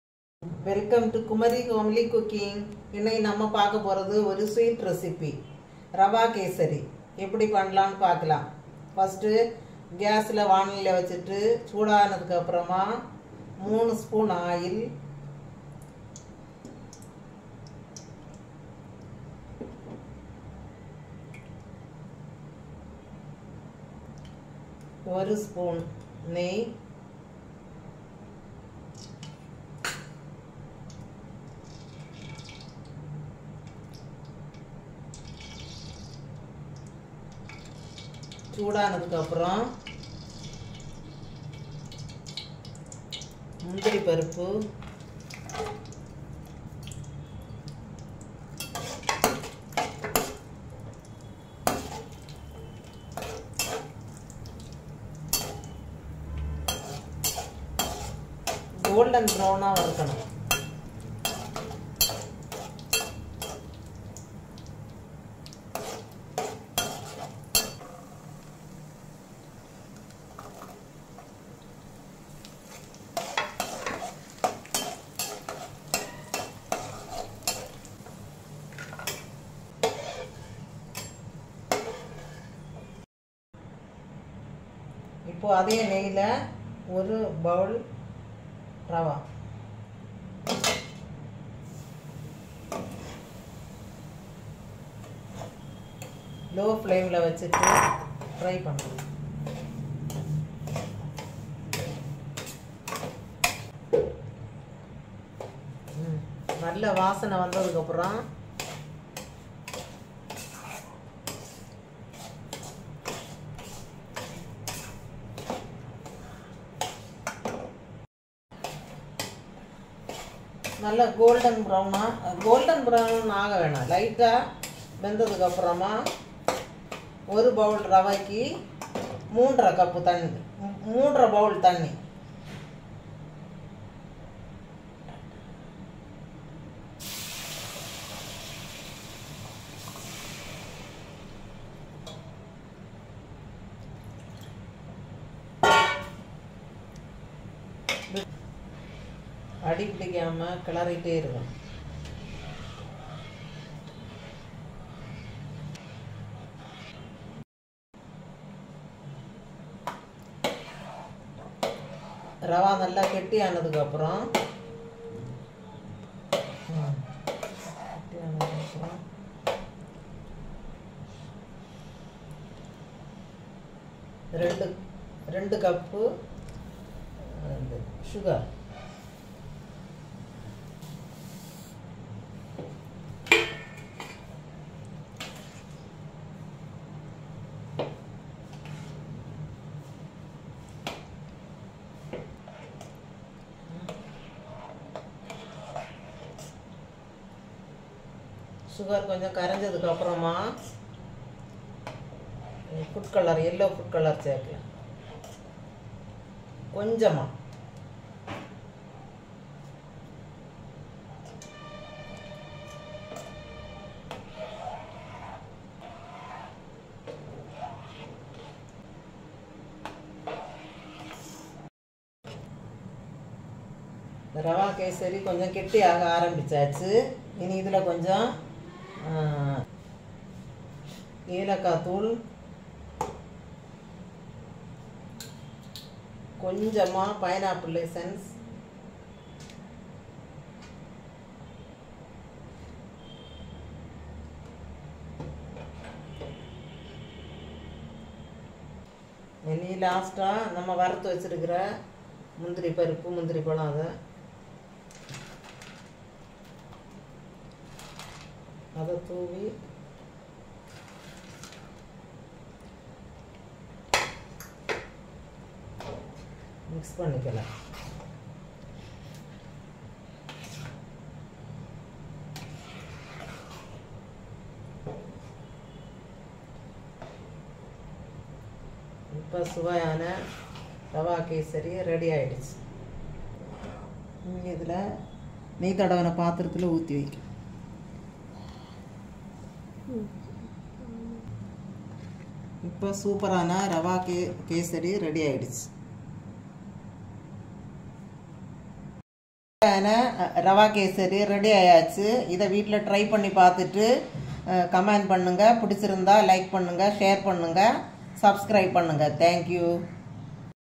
How to make Simple Pineapple Flavour Kesari
How to make simple pineapple flavour kesari | Easy| Pineapple Flavour| Kesari| @KumariHomelyCooking